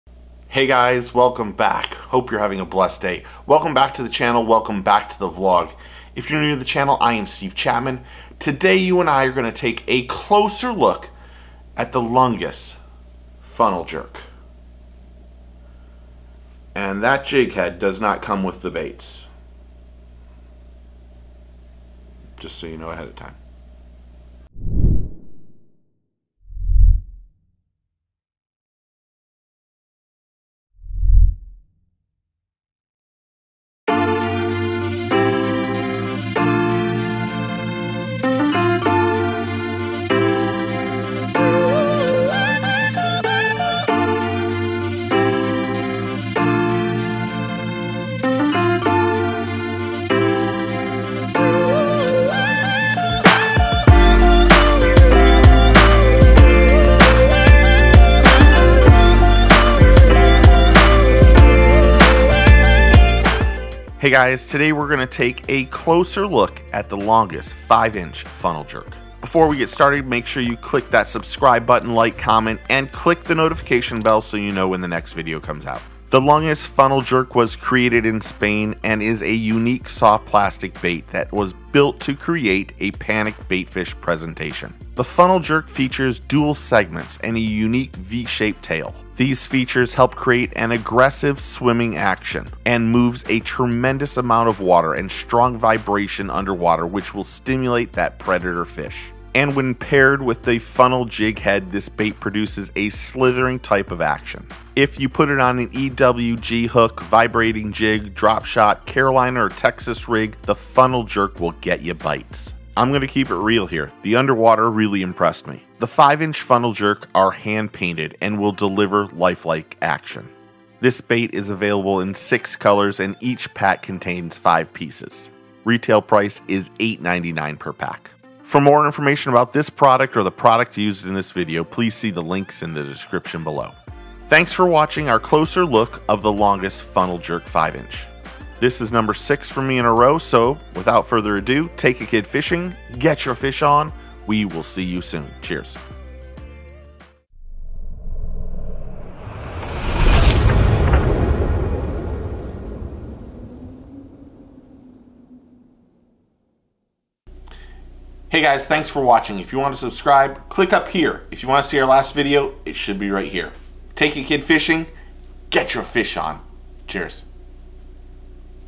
Closer Look at the Longas Funnel Jerk Soft Plastic Bass Fishing Jerkbait
New to the United States of America is the Longas Funnel Jerk 5" soft plastic bass fishing jerkbait.  This bait is made in Spain and built and designed to have a panicked bait fish presentation. This dual segmented body and V shaped tail give this soft plastic bass fishing jerkbait great action in the water to trigger aggressive bass strikes.